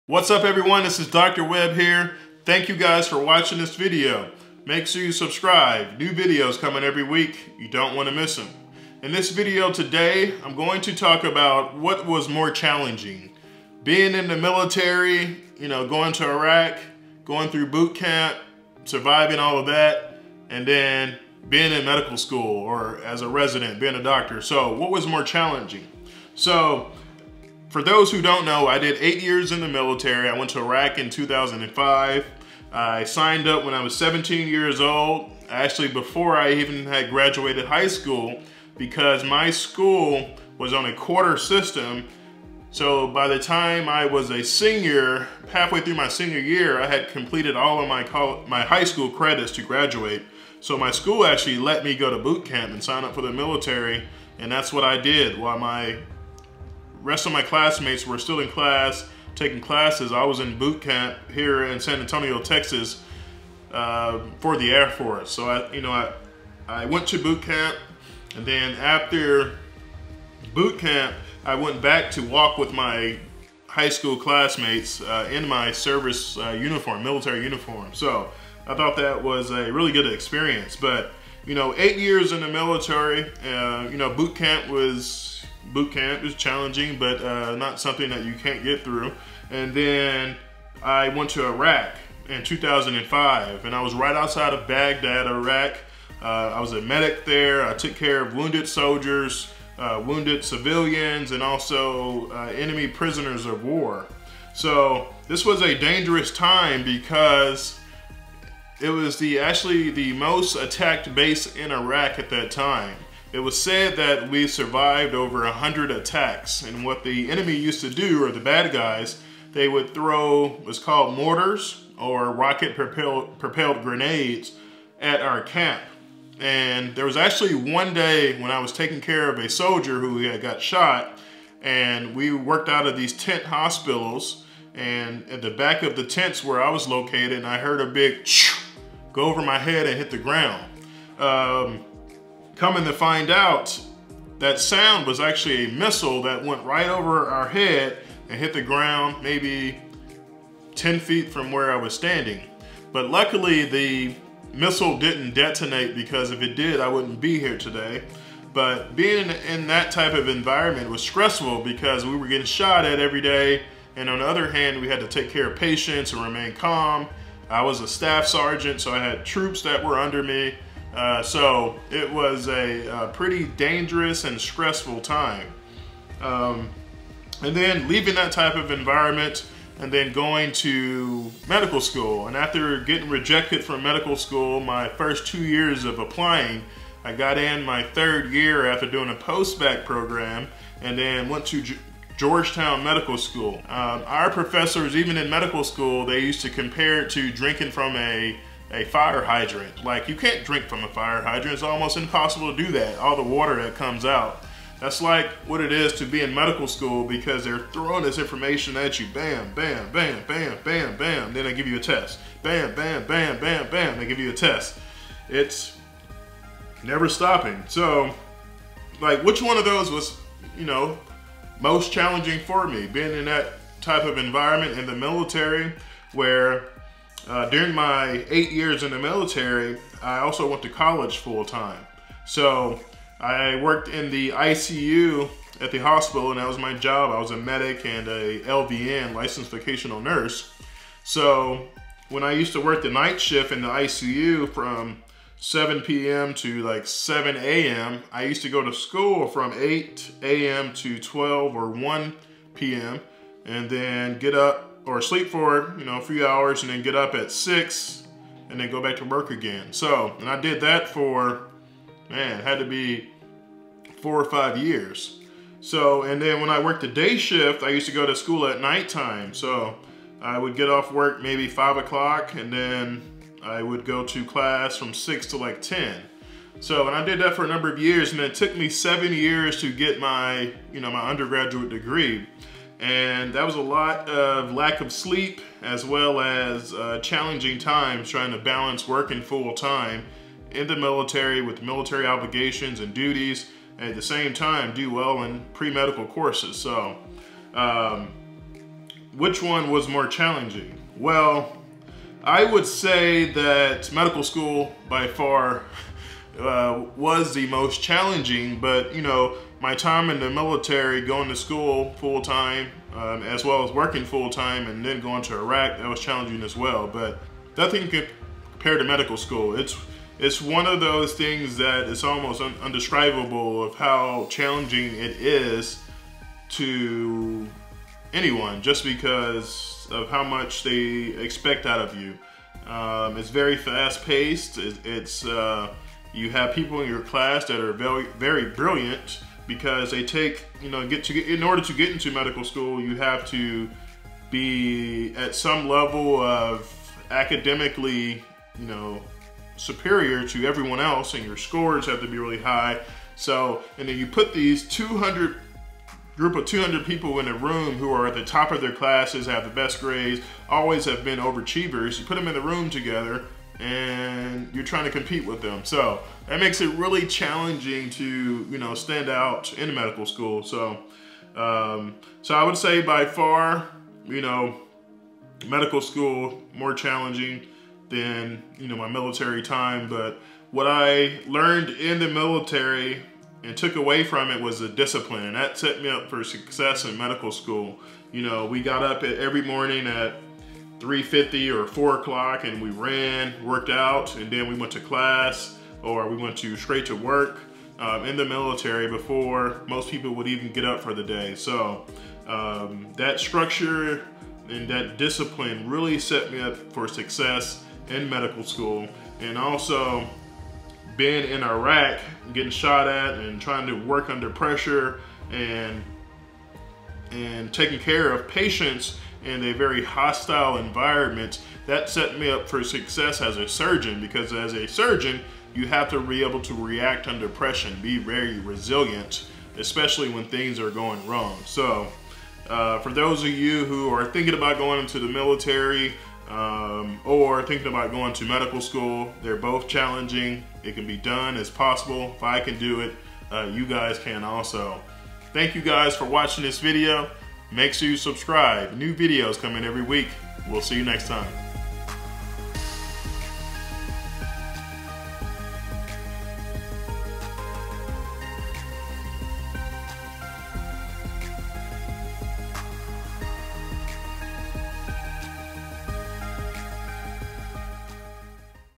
Military vs Medical School-Which one was more challenging?
In this video, Dr. Webb talks about what was more challenging: his 8 years in the military or 4 years of medical school!

1/2-Inch Extra Thick Exercise Mat with Carrying Strap